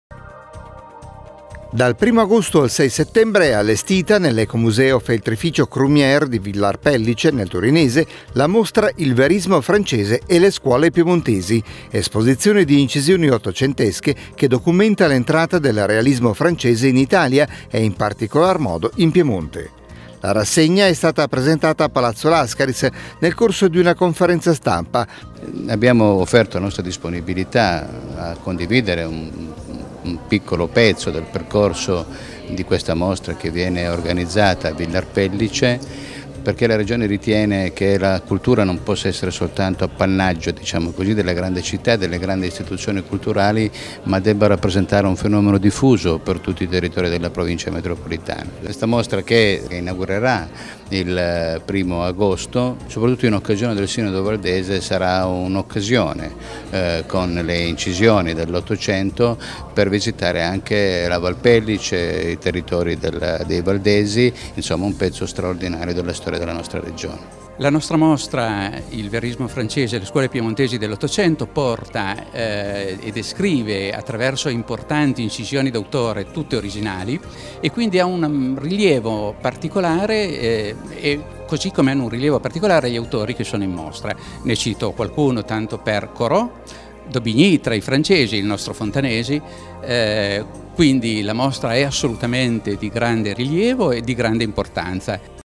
Il verismo francese e le scuole piemontesi
Dal 1° agosto al 6 settembre è allestita, nell’Ecomuseo Feltrificio Crumière di Villar Pellice, nel torinese, la mostra "Il Verismo francese e le Scuole piemontesi", esposizione di incisioni ottocentesche che documenta l’entrata del Realismo francese in Italia e, in particolar modo, in Piemonte. La rassegna è stata presentata a Palazzo Lascaris nel corso di una conferenza stampa, al termine della quale si sono esibiti Elena Bakanova, soprano, e Andrea Boccaletti, pianista.